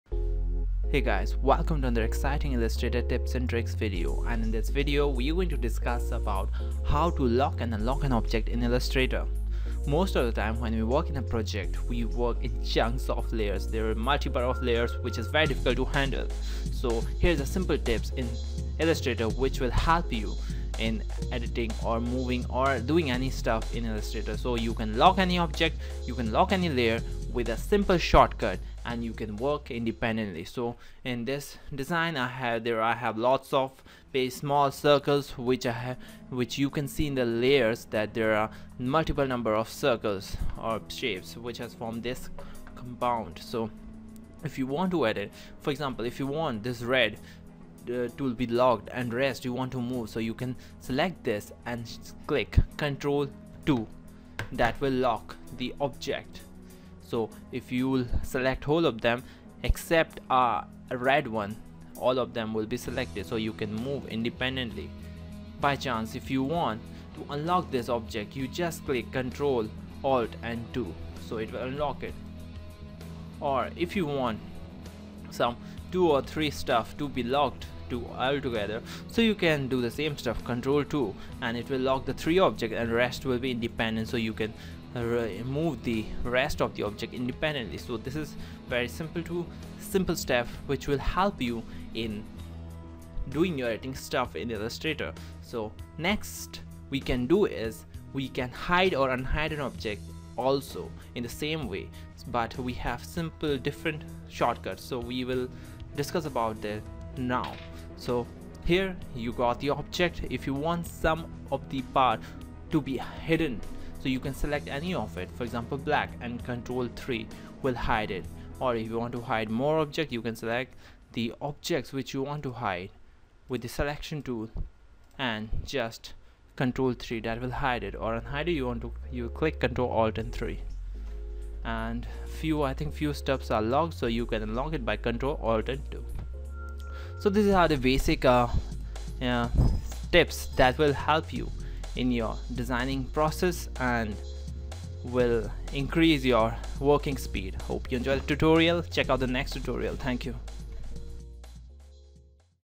LOCK-UNLOCK & HIDE-UNHIDE - ILLUSTRATOR TIPS 17
Here is another ILLUSTRATOR TIPS on Lock Unlock and Hide Unhide Objects.
 Hide unwanted objects by using Command + 3. Unhide all using Command + Alt + 3. This is similar to Lock/Unlock with invisibility.
Avoid accidental dragging of objects by locking them using Command + 2. Unlock them all using Command + Alt + 2

▲Gumroad Store!

▲Creatnprocess Channel!